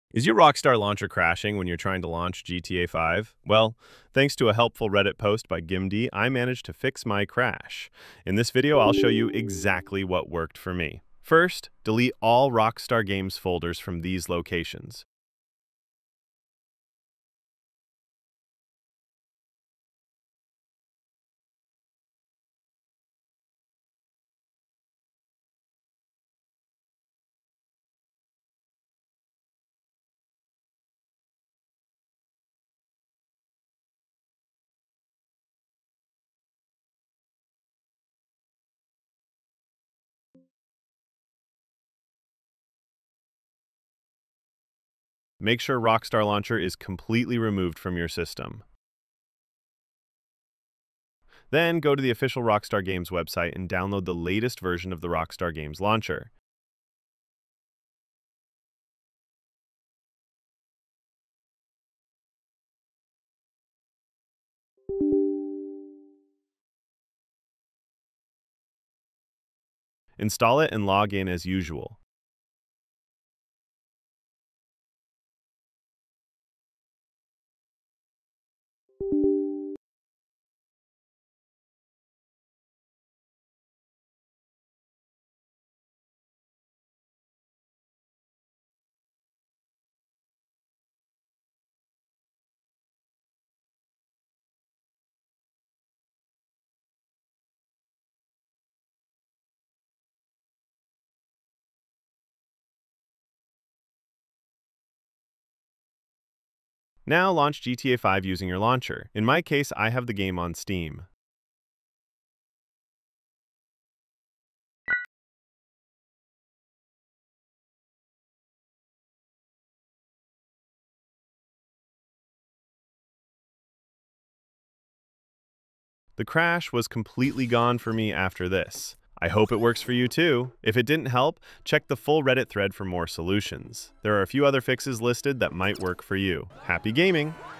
Rockstar launcher Crash fix | 2025
This is a quick fix for rockstart launcher keeps crashes on startup.
Time stamps -
0:00 - Fix
2:05 - End Note!
My PC specification :
CPU : Ryzen 7 7700
RAM : 16GB DDR5
GPU : NVIDIA 3070 8gb